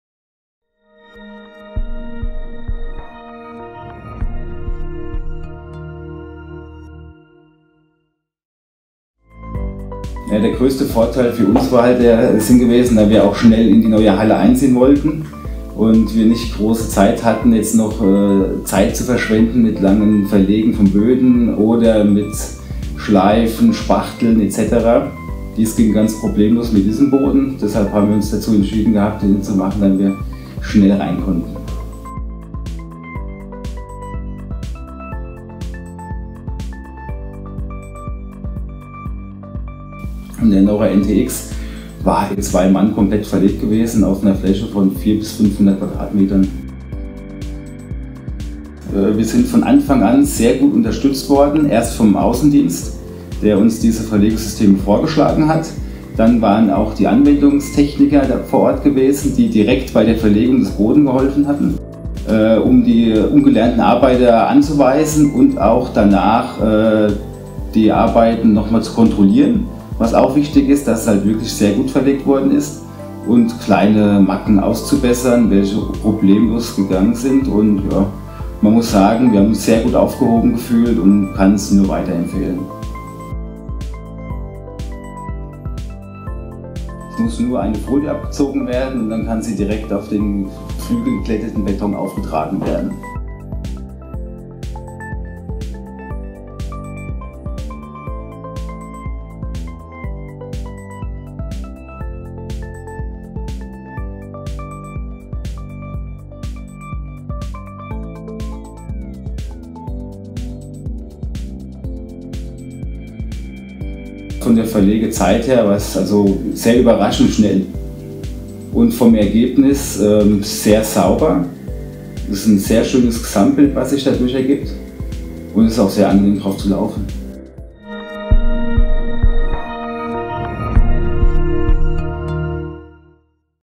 Nieuwbouw productiehal en magazijn „SanaCare“, Alsbach-Hähnlein, Duitsland
norament® nTx direct op betonnen vloeren
Betrouwbaar en snel, ook bij restvocht